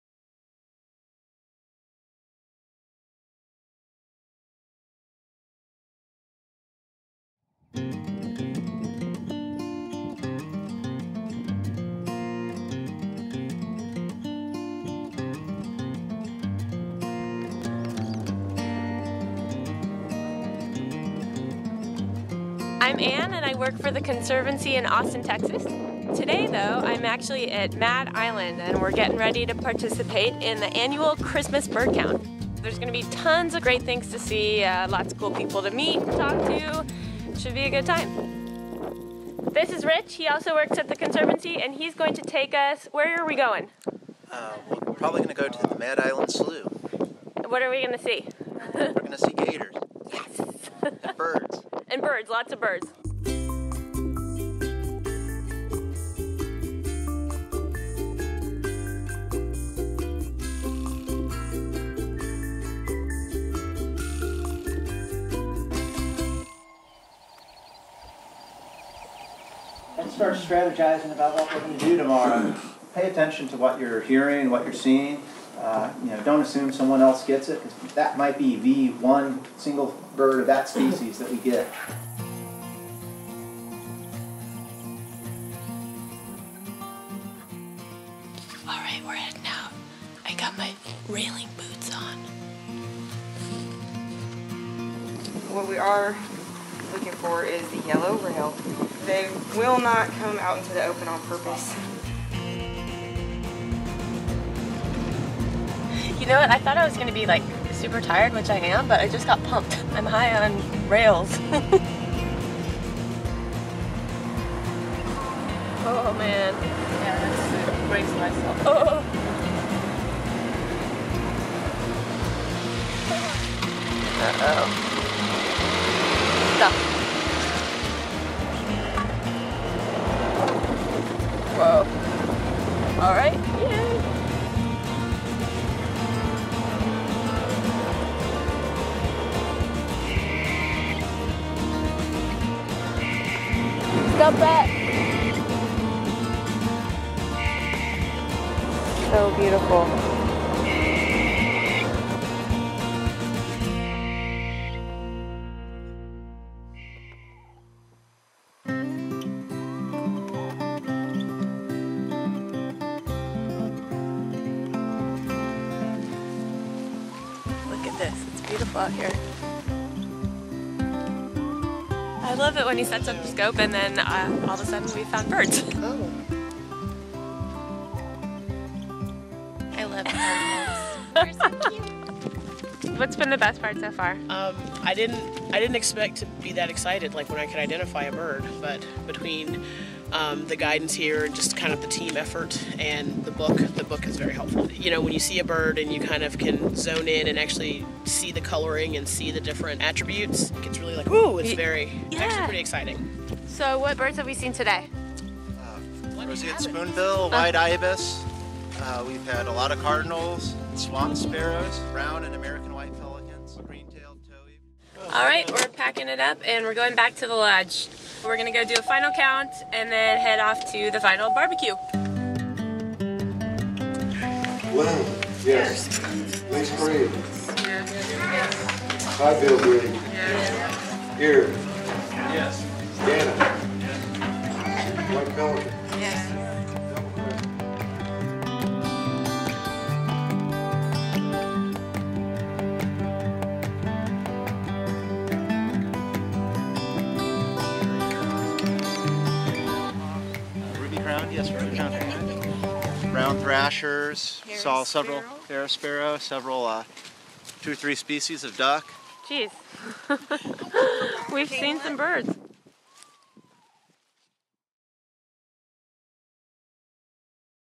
Mad Island Preserve Bird Count
Travel along with the Conservancy's Anne Zuparko as she participates in the annual Audubon Christmas Bird Count at Mad Island Marsh Preserve. This count has been ranked #1 in the nation for 14 years, and this year 244 species were spotted! Bird counting is more than just relaxing with a pair of binoculars—participants in this count embarked on 4:00 a.m. ATV trips through the mud, got up close with alligators and more!